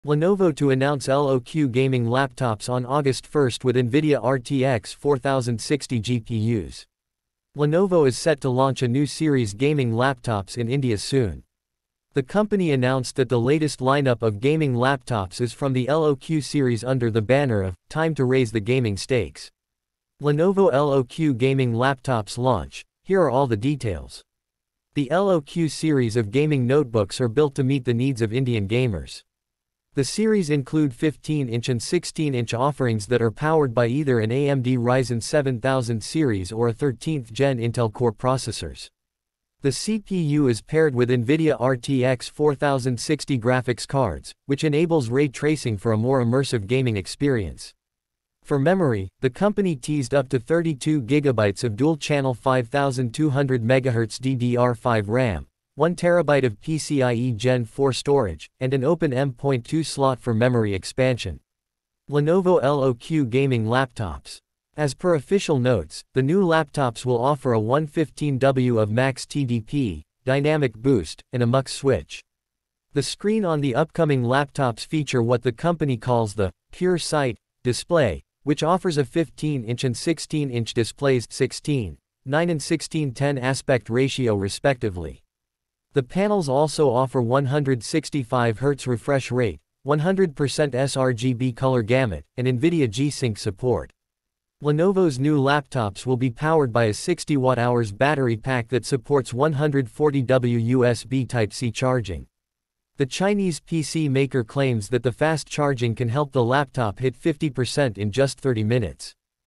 Lenovo to announce LOQ gaming laptops on August 1 with Nvidia RTX 4060 GPUs
Lenovo is set to launch a new series gaming laptops in India soon. The company announced that the latest lineup of gaming laptops is from the LOQ series under the banner of “Time to Raise the Gaming Stakes”.